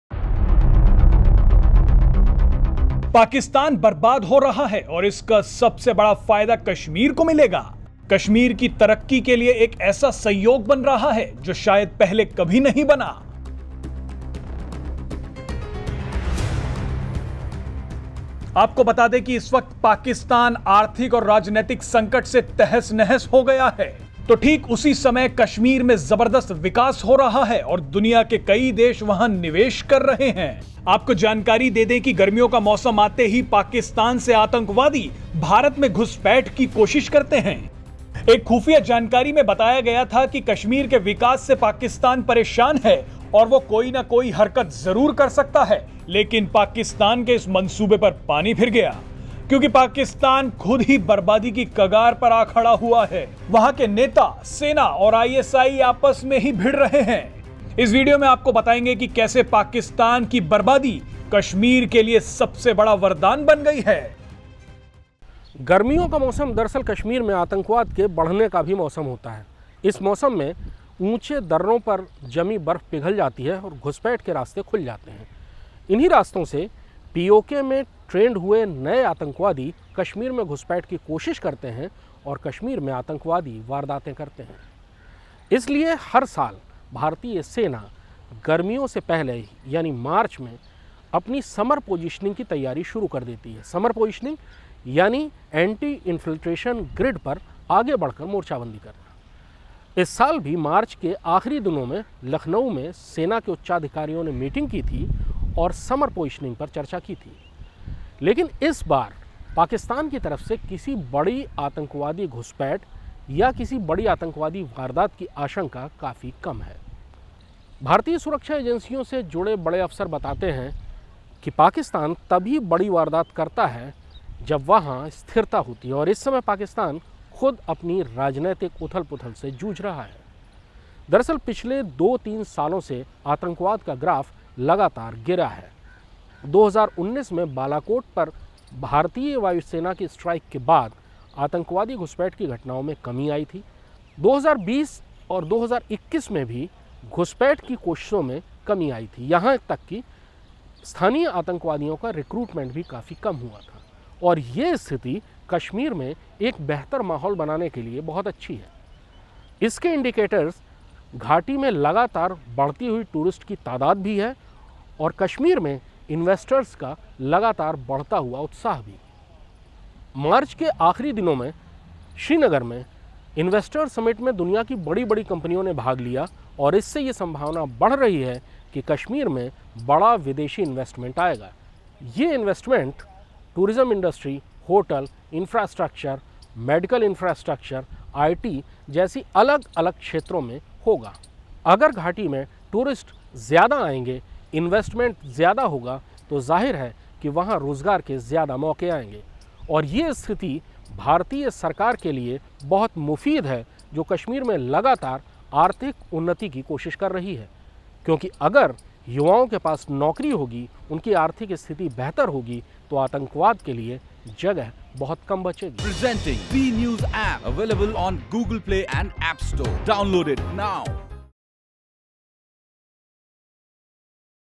पाकिस्तान की बर्बादी बनी कश्मीर के लिए वरदान, हुआ गजब खेल | Jammu Kashmir | Industrial Investment
पाकिस्तान बर्बाद हो रहा है और इसका सबसे बड़ा फायदा कश्मीर को मिलेगा. कश्मीर की तरक्की के लिए एक ऐसा संयोग बना है जो शायद पहले कभी नहीं बना. इस वीडियो में जानिए क्या है वो संयोग.